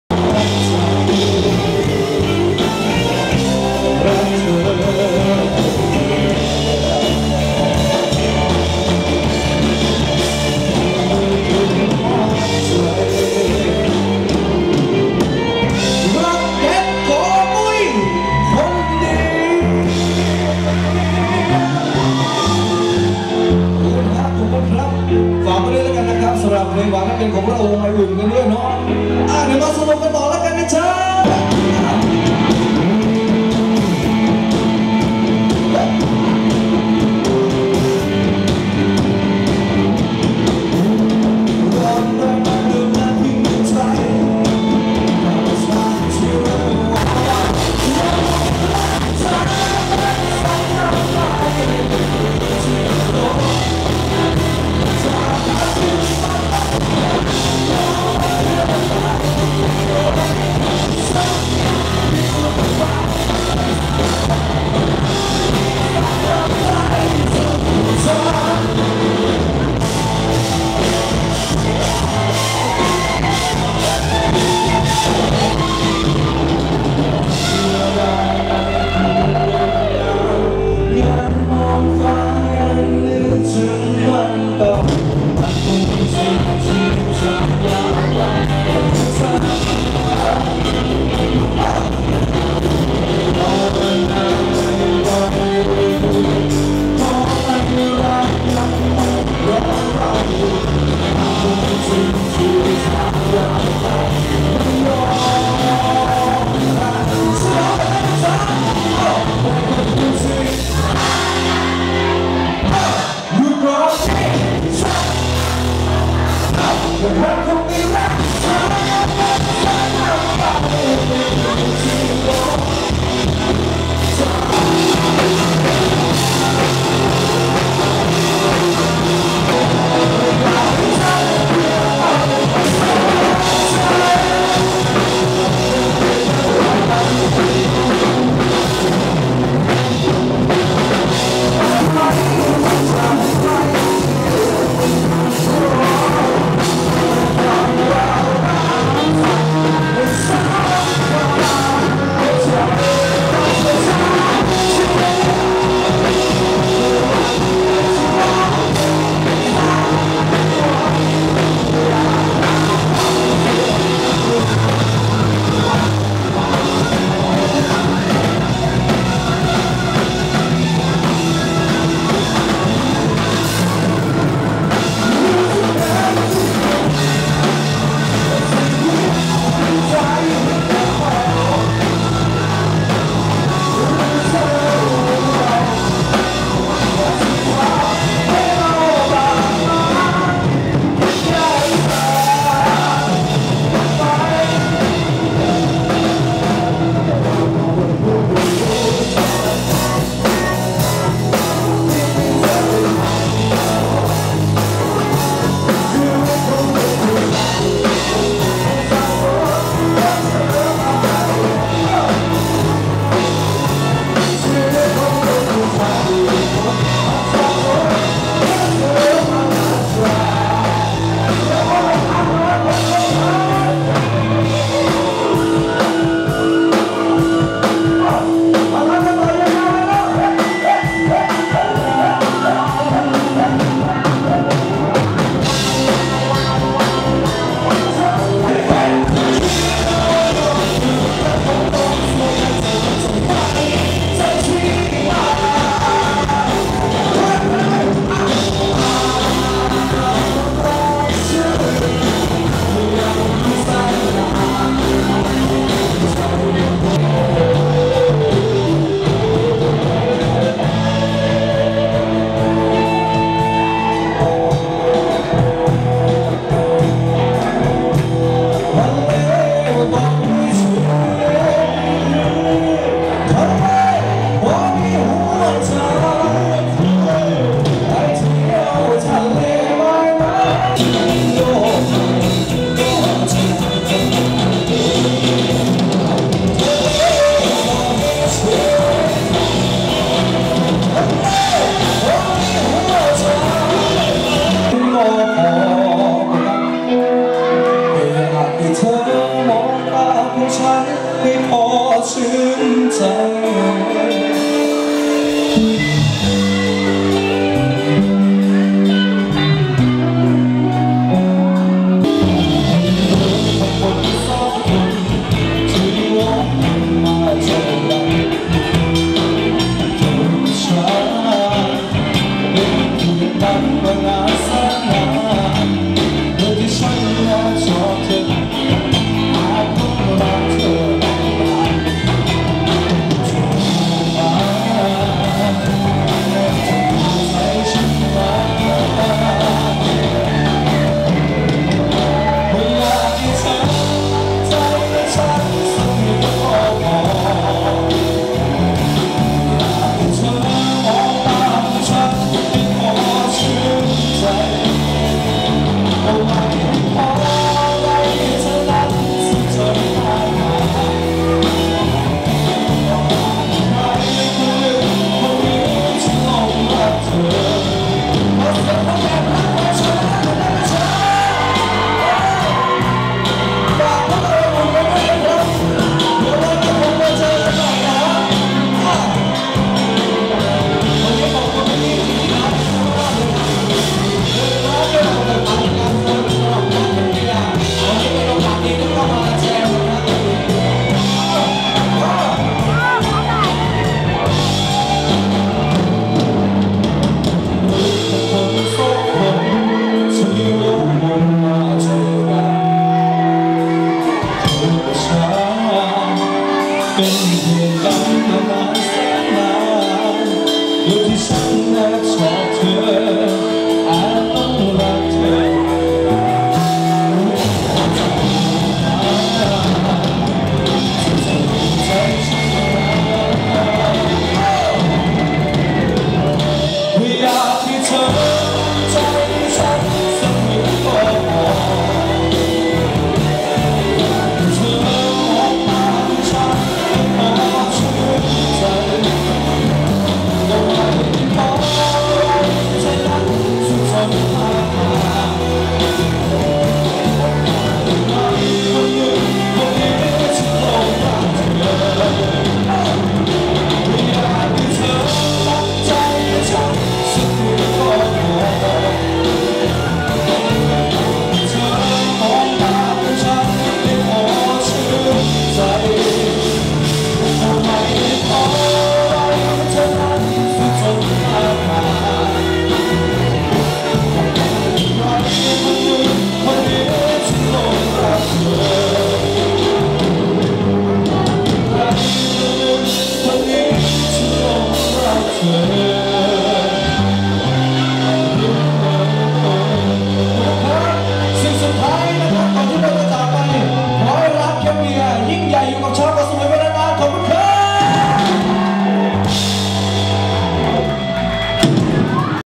วง ไออุ่น แคมป์เบียร์ เกาะสมุย
ฝันเล็กๆที่ยิ่งใหญ่สำหรับวงไออุ่น จาก ต้นกล้าฅนเพลง เวทีที่ร้าน แคมป์เบียร์ เกาะสมุย จากวงไออุ่น เมล็ดพันธ์ใหม่ที่เริ่มปักธงที่เกาะสมุย ติดต่องานแสดงที่